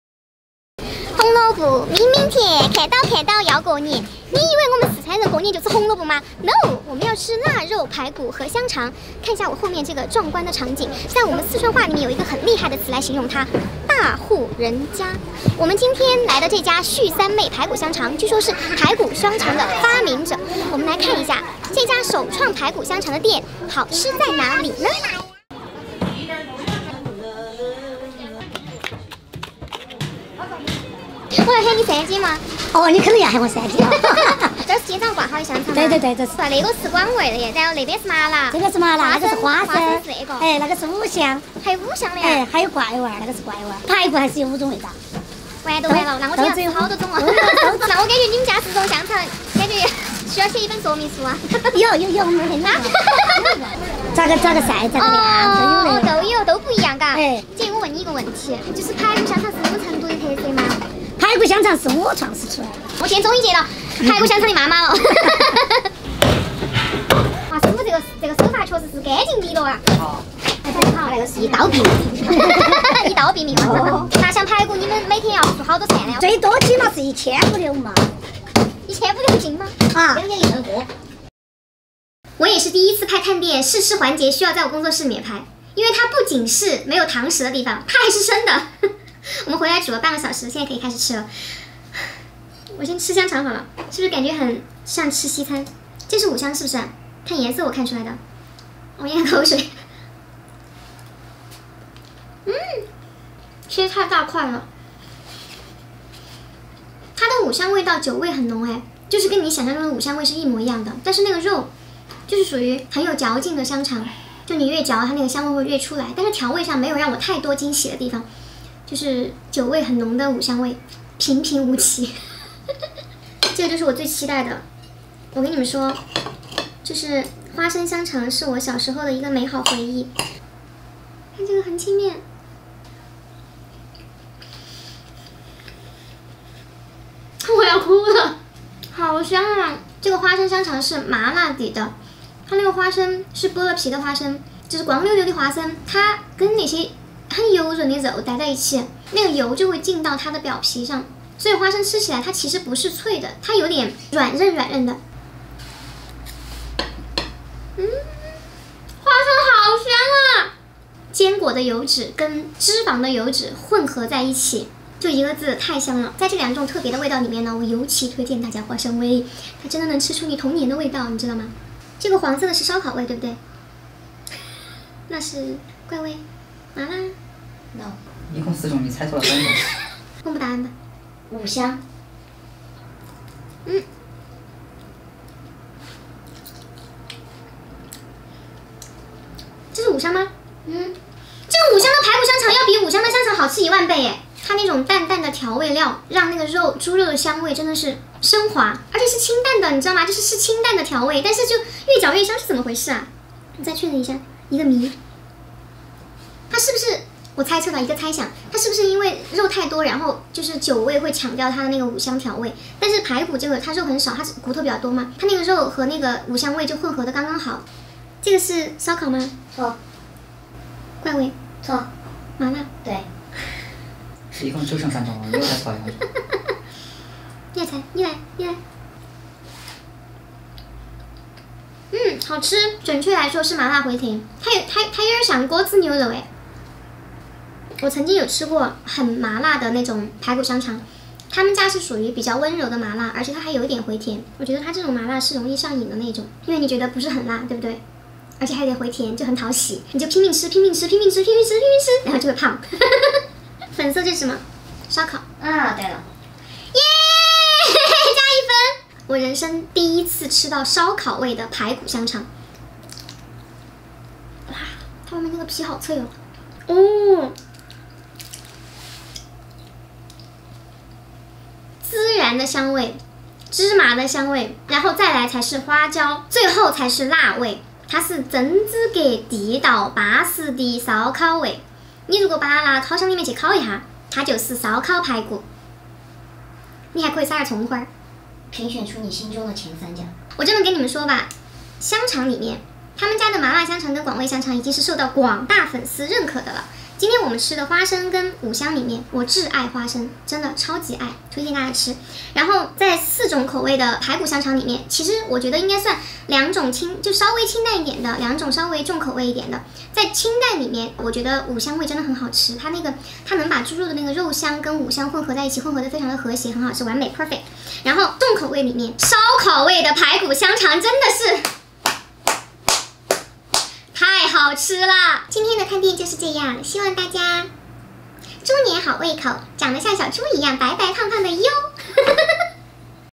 深藏在菜市场的年货美味，2天卖1吨的胥三妹香肠
胥三妹香肠
地址：金科南三路南段土桥农贸市场内